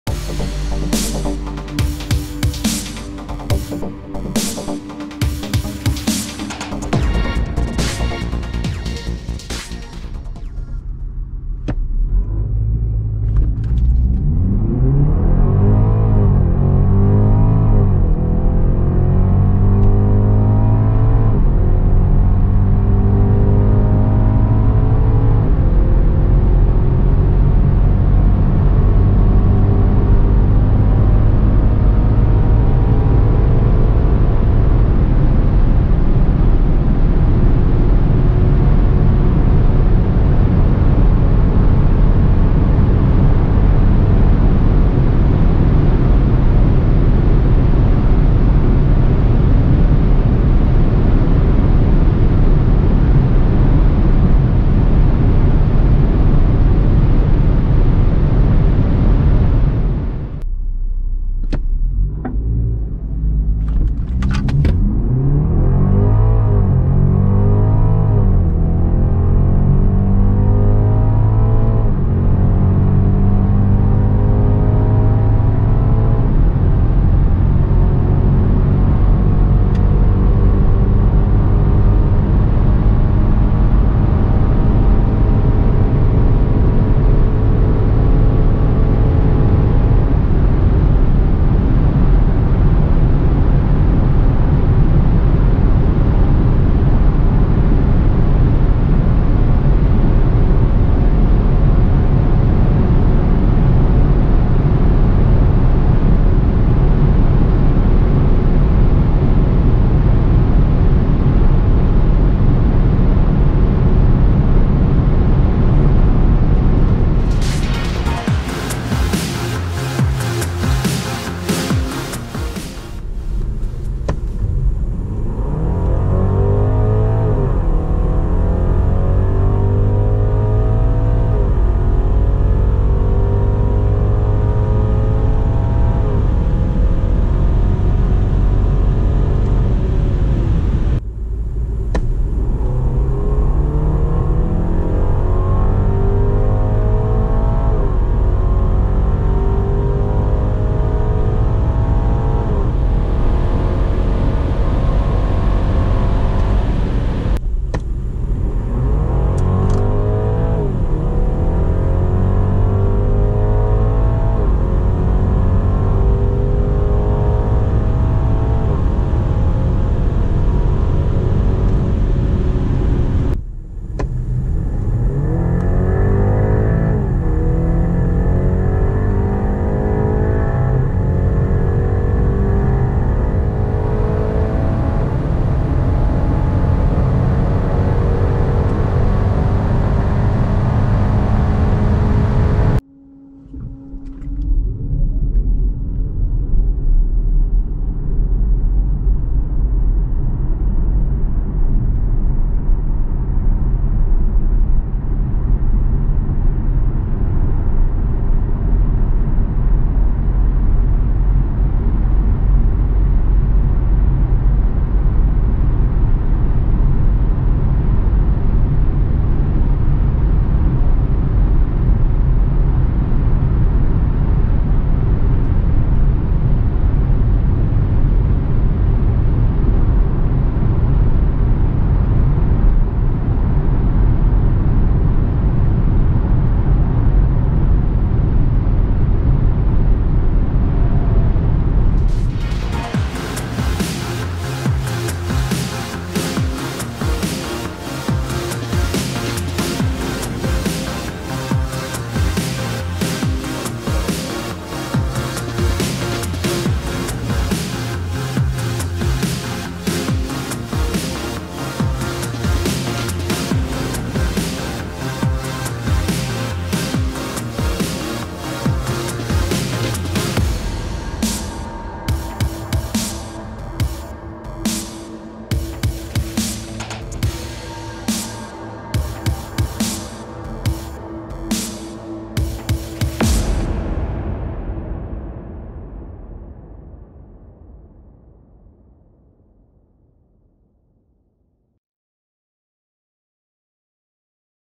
BMW 330e xDrive 2021 acceleration: 0-60 mph, 0-100 km/h 0-100-200 km/h top max speed vmax : 1001cars
This is the acceleration (beschleunigung) of BMW 330e xDrive Touring (2021/2022) Plug-In Hybrid 2020/2021 (PHEV): 2.0 l R4 turbo petrol engine + electric, 215 kW, 292 HP, 420 Nm, xDrive, 8-speed Steptronic from 0-60 mph, 0-100 km/h, 0-140 km/h and 0-200 km/h (up to top/max speed). I checked drag time with empty battery also.

Results for BMW 330e xDrive PHEV Touring:
0-60 mph: 5.3 s
0-100 km/h in 5.7 s
0-140 km/h in 10.1 s
0-100 mph in 13.4 s
0-200 km/h in 22.0 s
top measured GPS speed: 220 km/h (136.5 mph)

80-120 km/h in 3.5 s
70-140 km/h in 6.9 s
60-100 mph in 8.2 s
100-200 km/h in 16.3 s

Electric Mode:
0-50 km/h in 4.1 s
0-60 mph in 12.2 s
0-100 km/h in 13.5 s
0-120 km/h in 20.1 s
0-140 km/h in 31.0 s